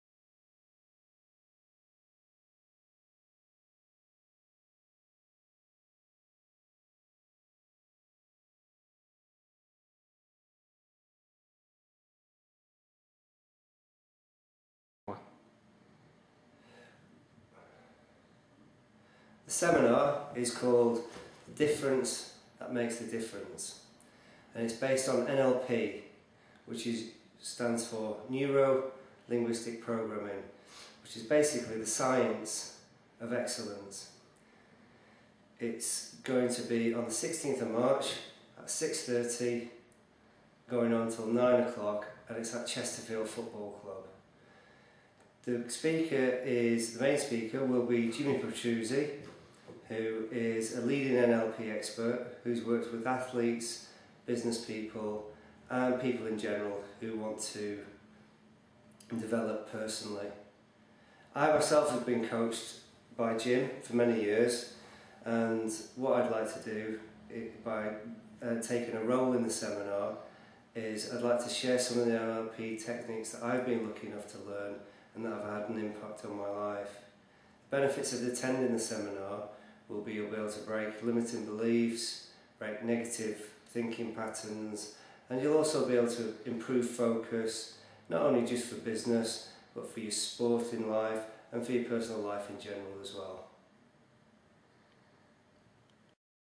Introduction to NLP
Jonathon Tinkler discusses the Difference that makes the Difference Seminar hosted in Chesterfield March 16th with Key note Speaker Jimmy Petruzzi